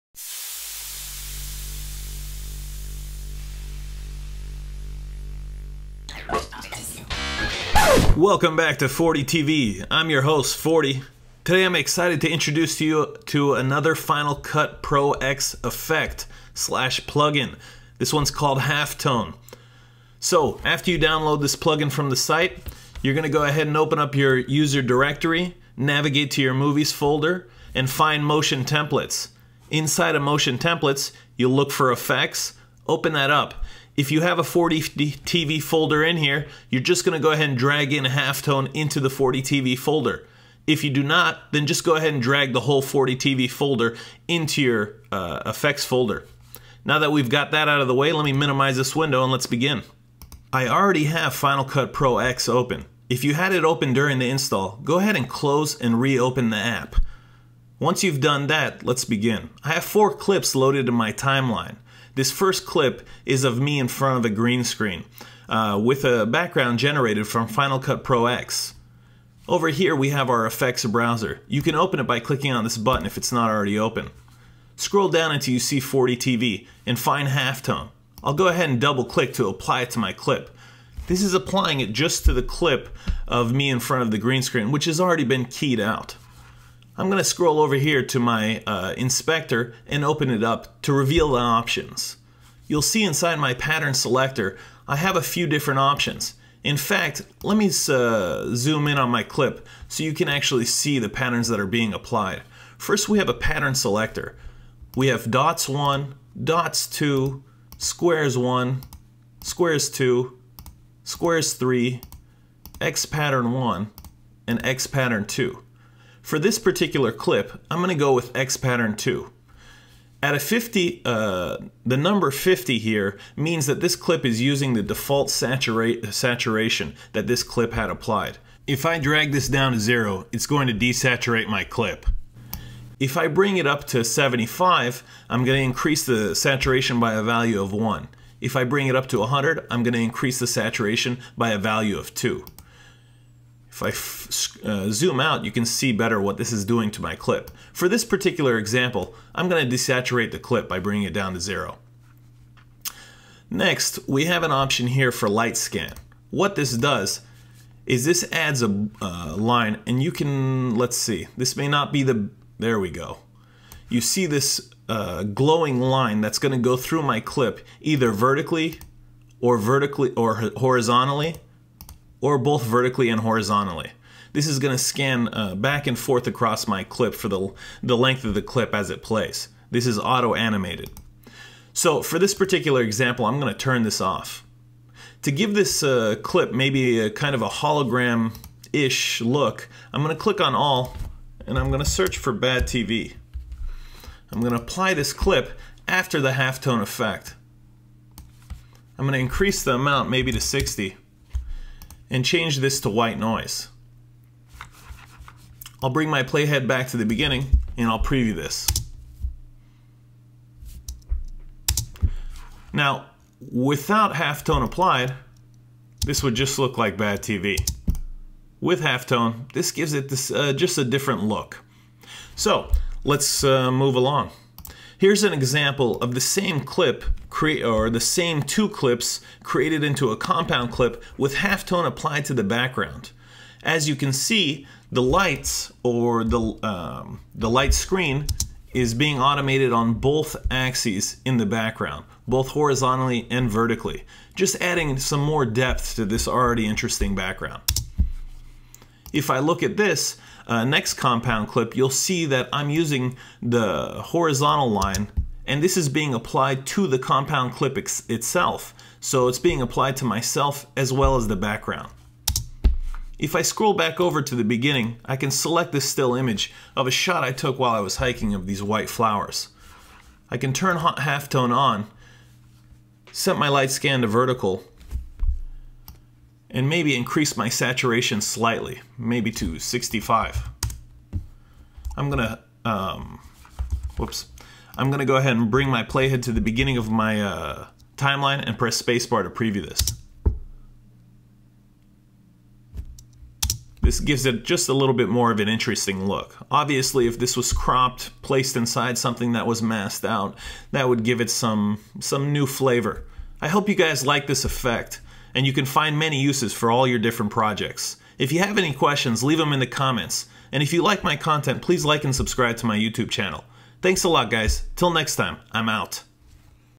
FCPX Effect - Halftone for Final Cut Pro X
Video Tutorials For Beginners-Advanced Using Final Cut Pro X (FCPX)

This video shows you how to use FordeeTV's Halftone Effect for Final Cut Pro X (FCPX Plugin).

The Halftone effect applies one of several patterns to your clip. These patterns include dots, squares and x's. You can control the way saturation is affected by the pattern against the original clip. You can also toggle an animated glowing scan light that traverses your clip either horizontally, vertically or both.